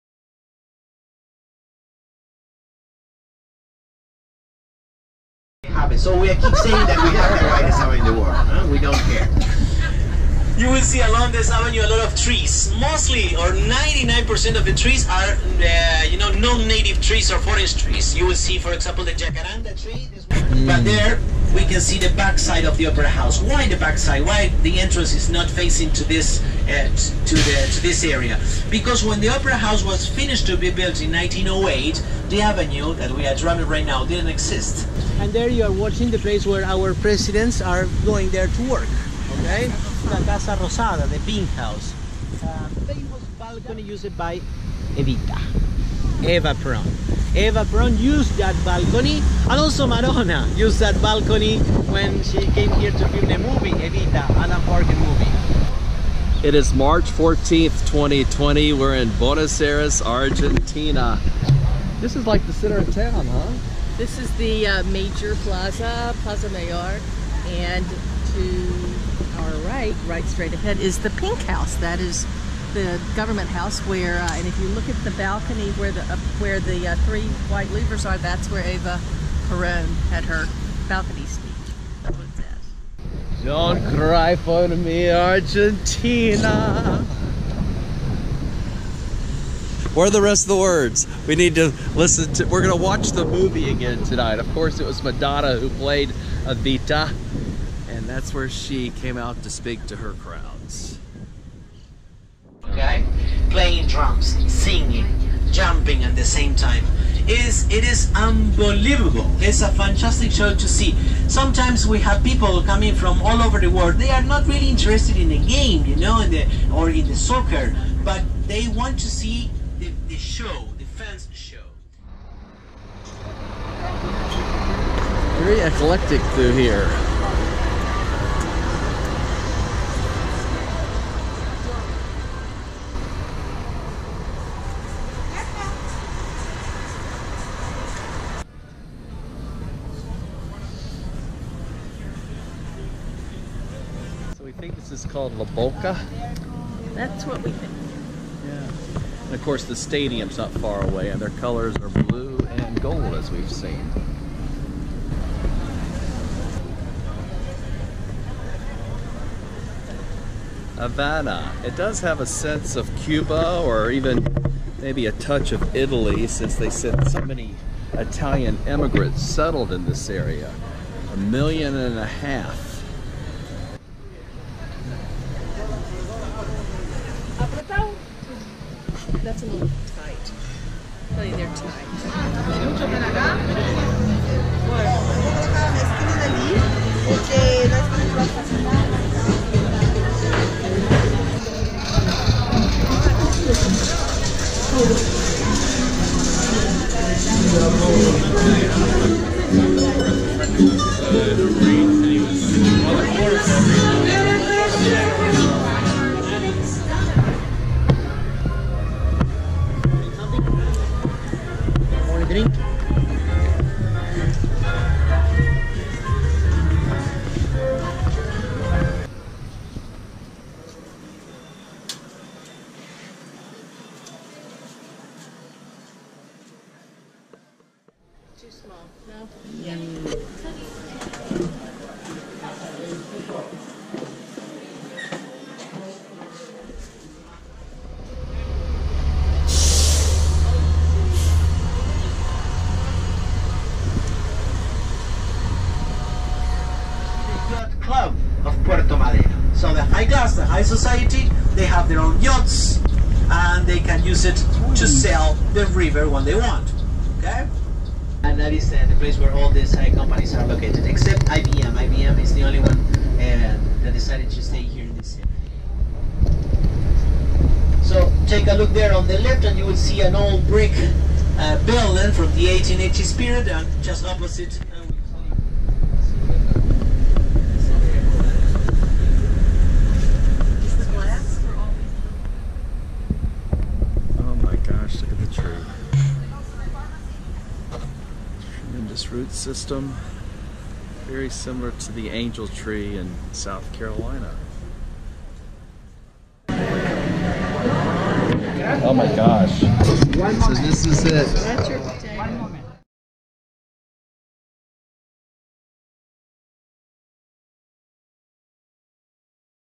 Buenos Aires Argentina
We visited the home of Evita and took in the lively atmosphere the city has to offer.